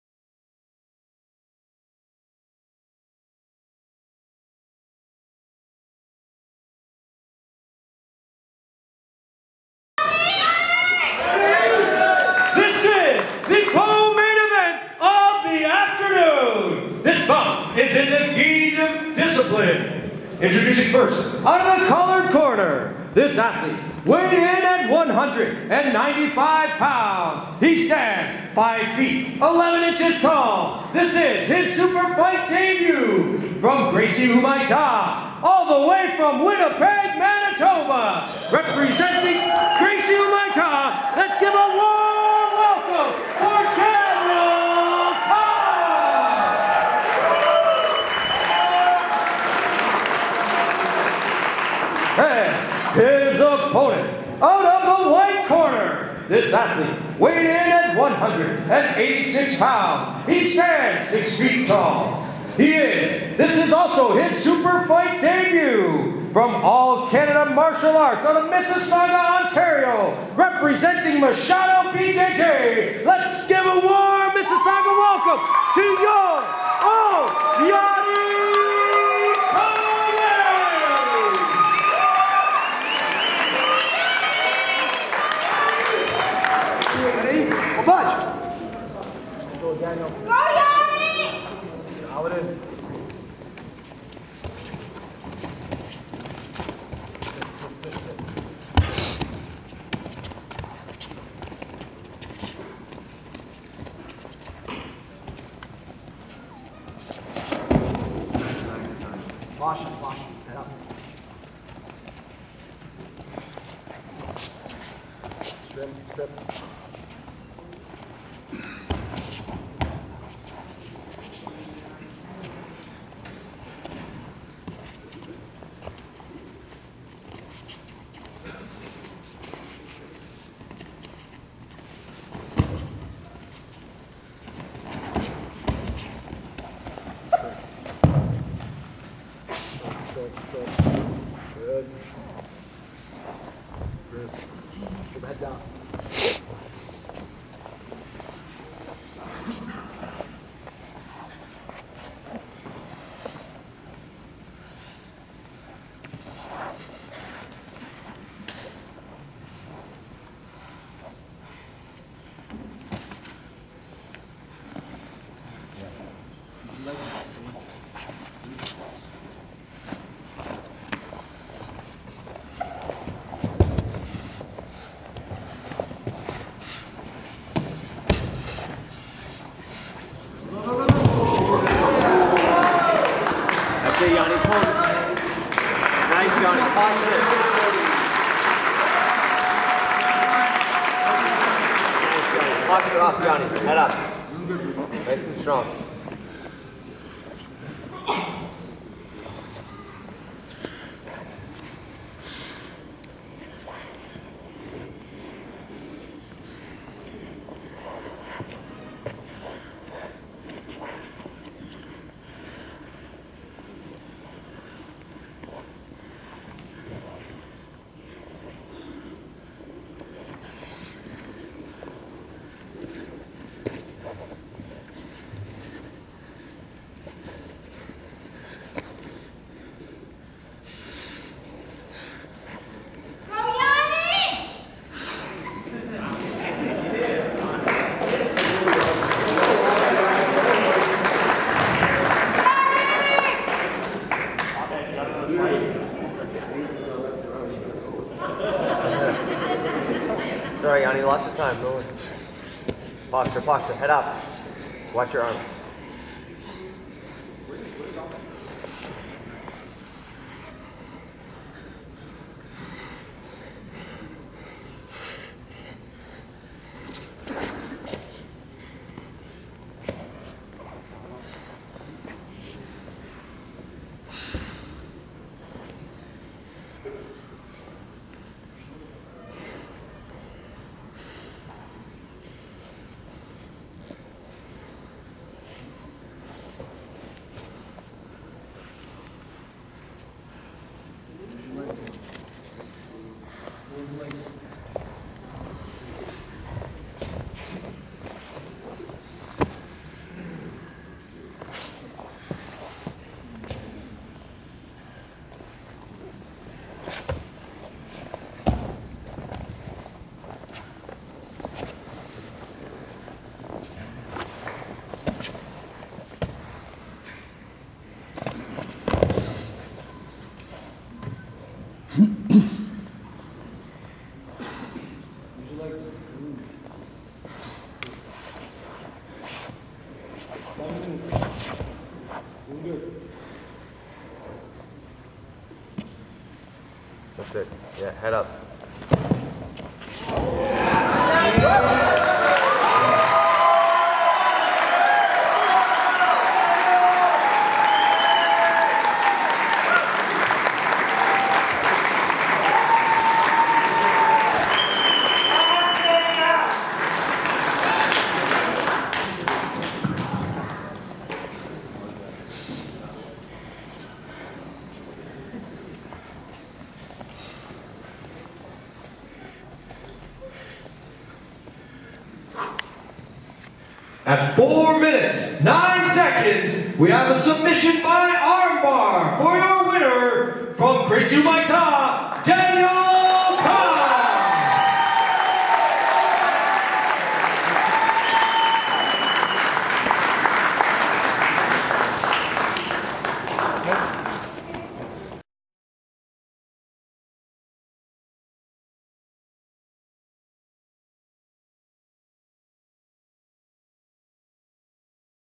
Daniel Cobb X Yanni Tome ( 2016 Canadian Sub Ex) GRAPPLERSPLANET.COM
Co-Main Event of the Black Belt SuperMatch.